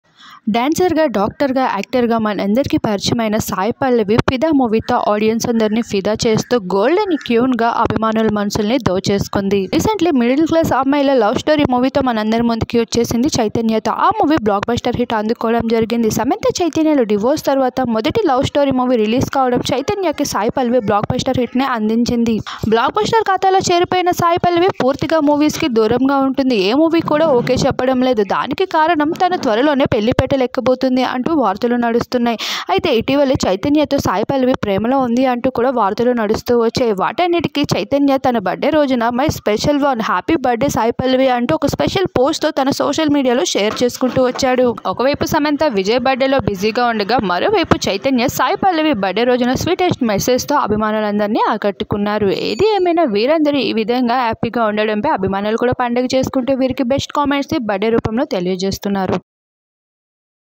నా స్పెషల్ వన్ హ్యాపీ బర్త్ డే సాయి పల్లవి అంటూ చైతన్య లేటెస్ట్ పోస్ట్//saipallavi chaitanya
స్పెషల్ వన్ హ్యాపీ బర్త్ డే సాయి పల్లవి అంటూ చైతన్య లేటెస్ట్ పోస్ట్//saipallavi chaitanya